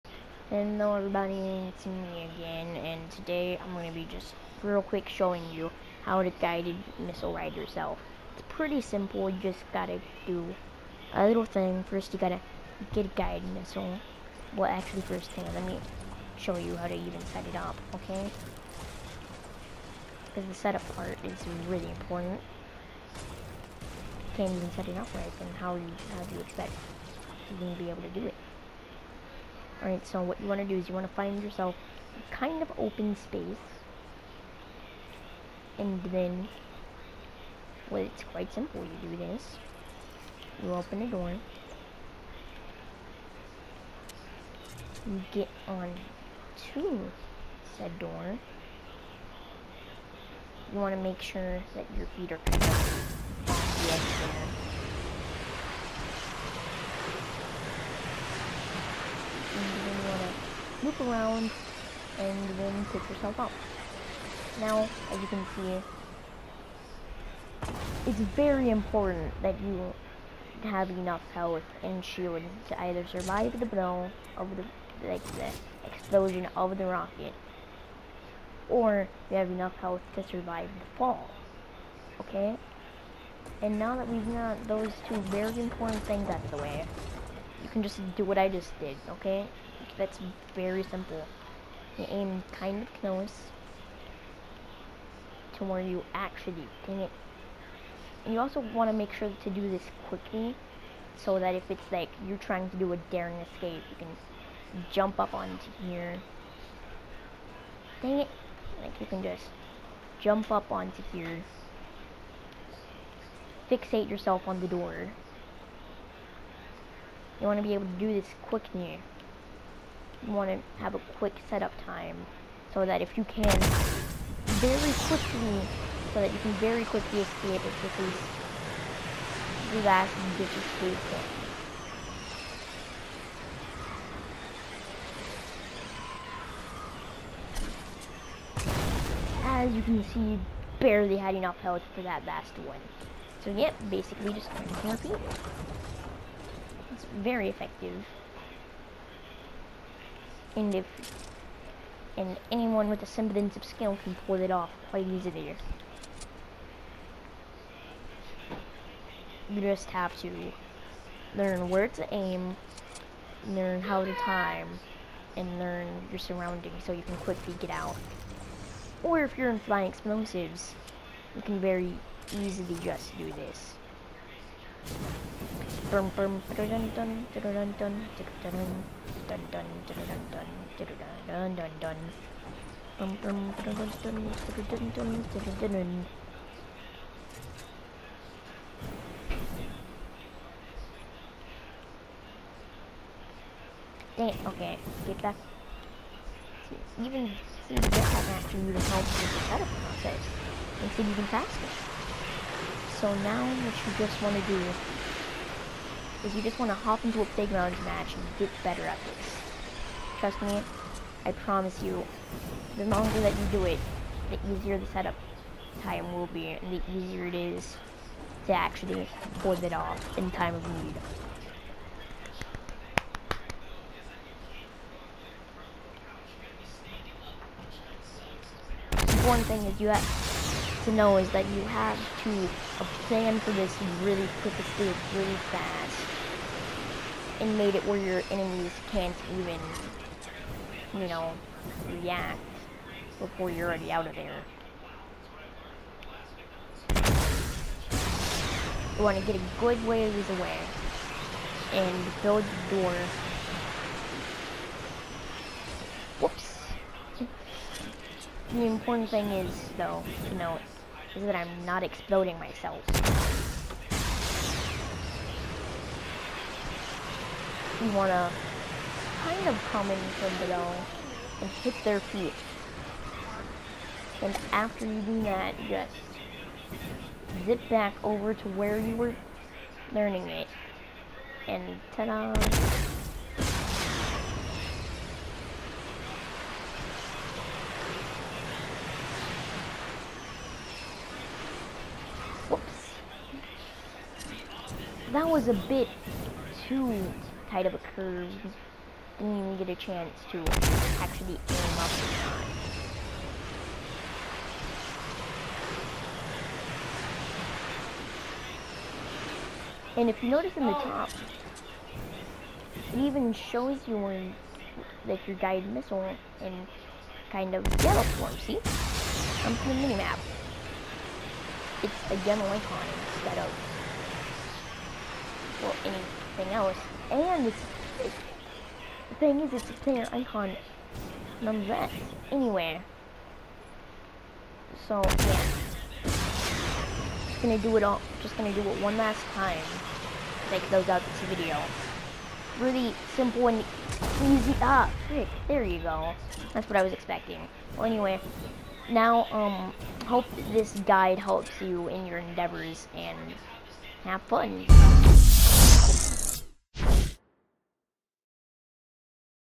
Dummy's Guide to Self-Rocket Riding using the Guide-d Missile
It's not hard to do, just practice